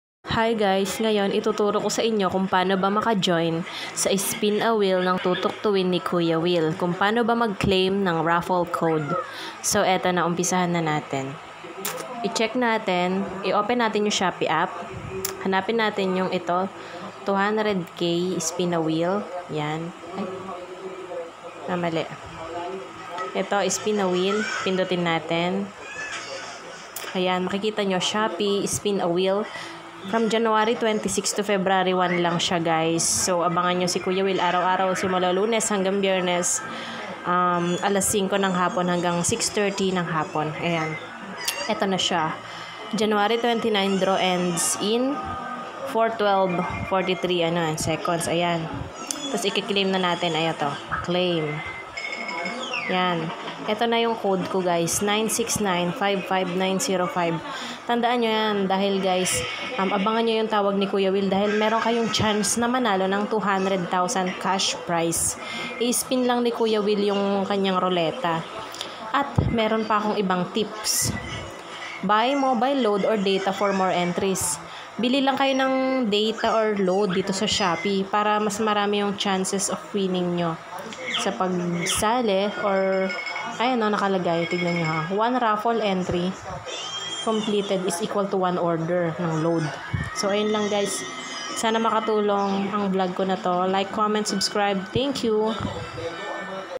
Paano manalo ng 200k sa Shopee Spin-A-Wheel | How to Claim the raffle code | Techniques
Paako mag claim ng code sa Shopee App para Manali ng 200k saa Spin-A-Wheel